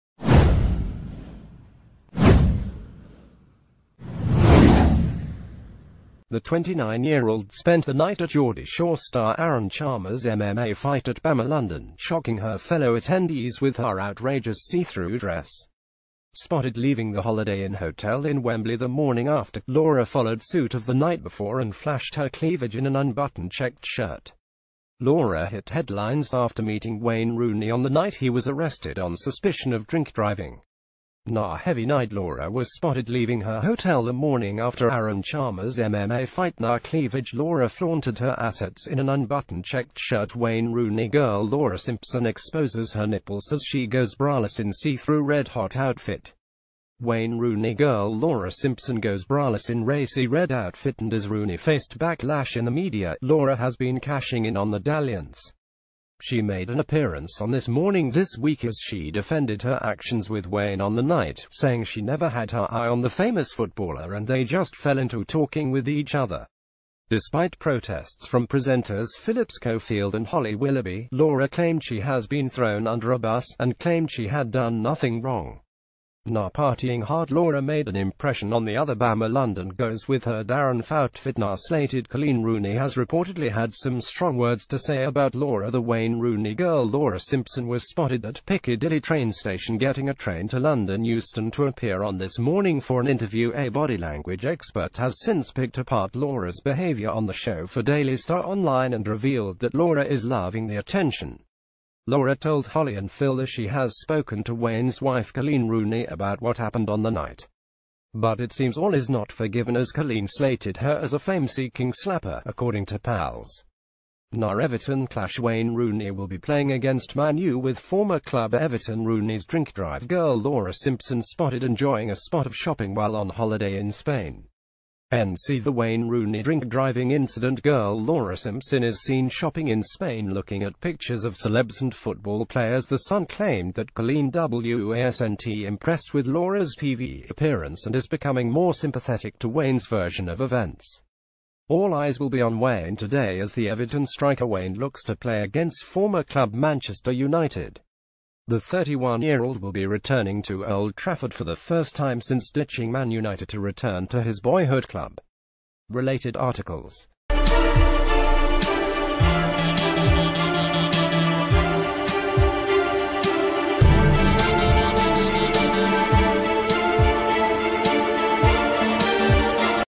Breaking News | Rooney girl laura simpson flashes assets leaving hotel as wayne returns to man utd
Rooney girl laura simpson flashes assets leaving hotel as wayne returns to man utd

The 29yearold spent the night at Geordie Shore star Aaron Chalmers MMA fight at BAMMA London, shocki...